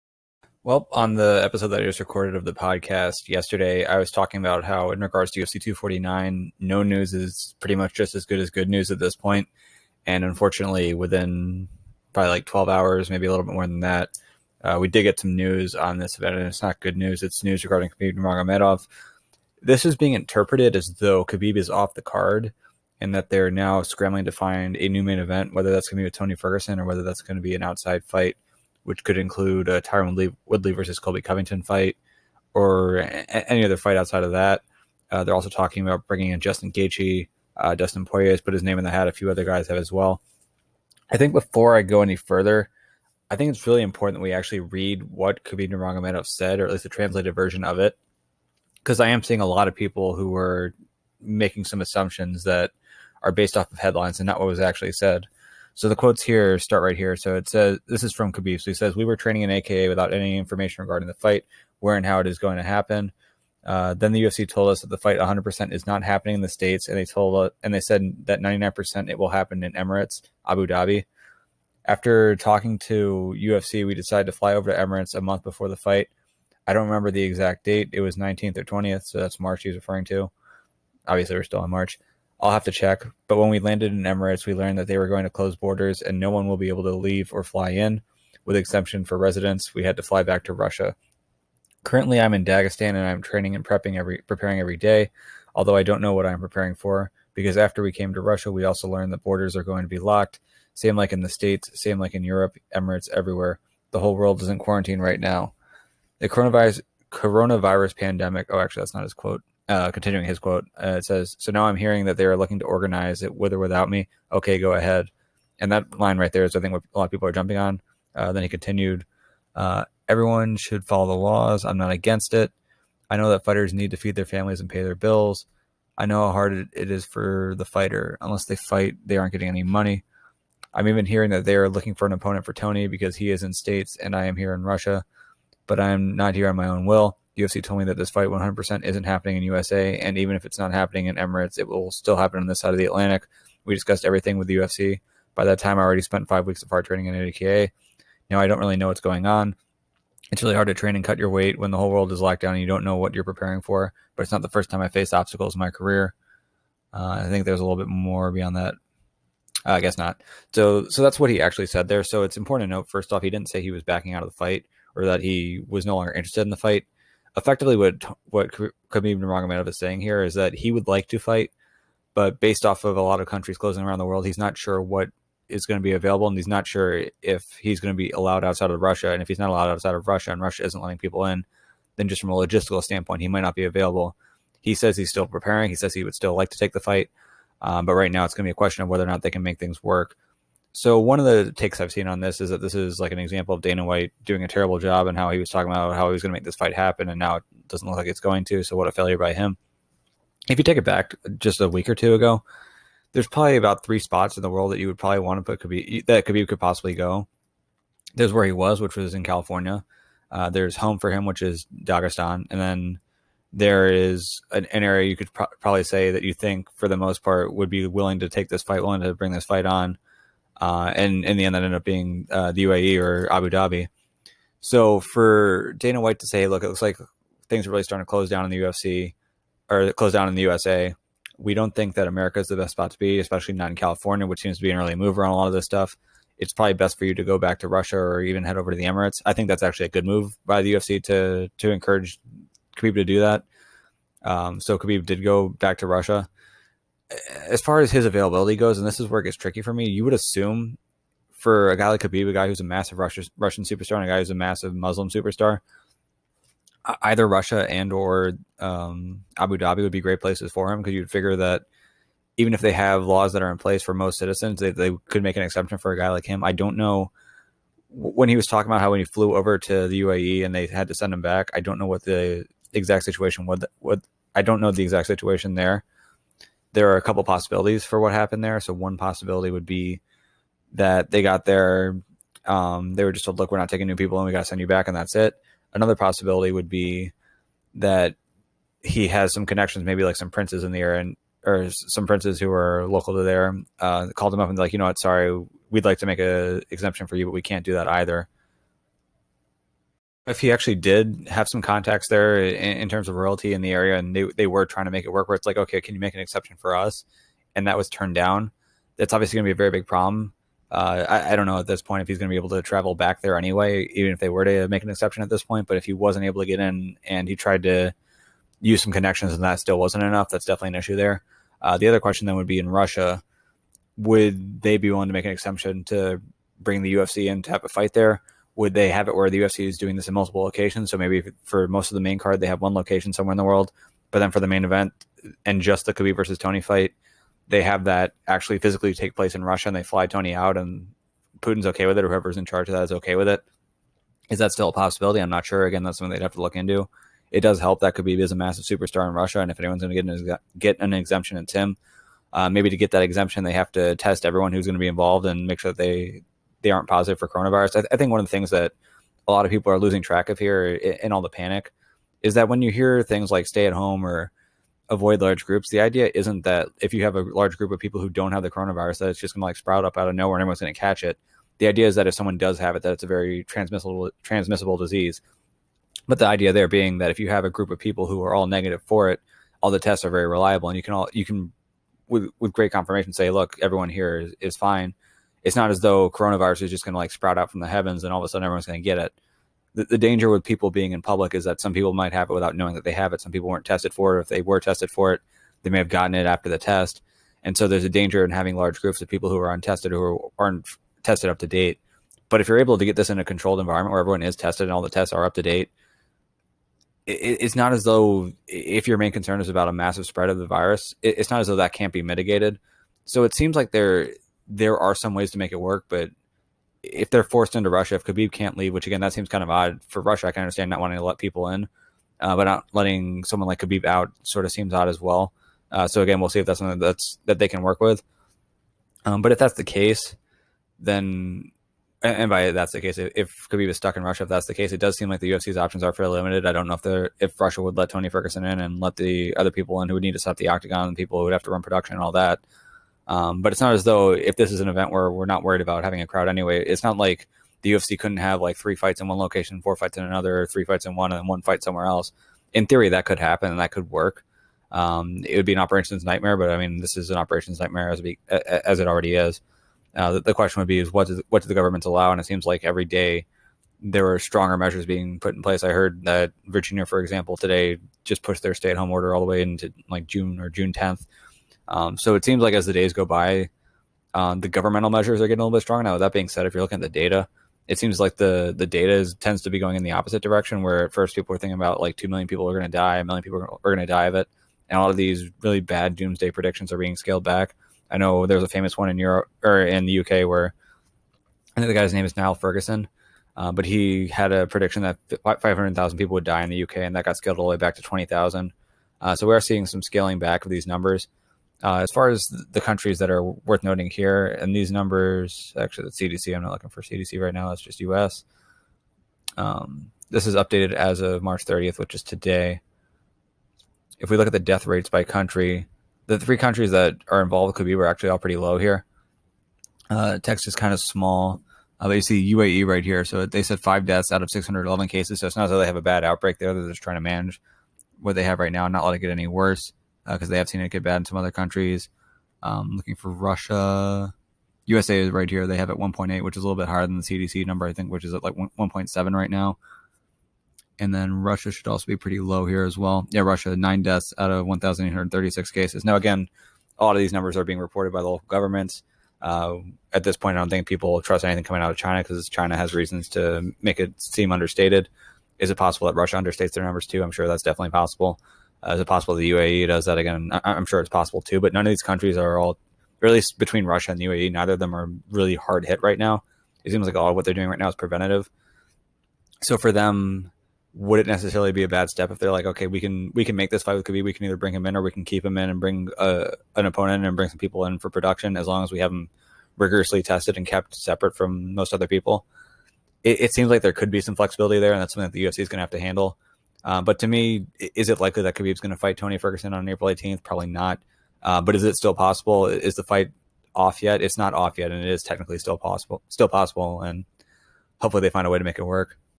Khabib Nurmagomedov In Severe Danger Of Being Pulled From UFC 249
In an interview this morning, Khabib Nurmagomedov painted a bleak picture that will likely end in him being off the UFC 249 main event. I recap what he actually said and discuss the implications in the video above.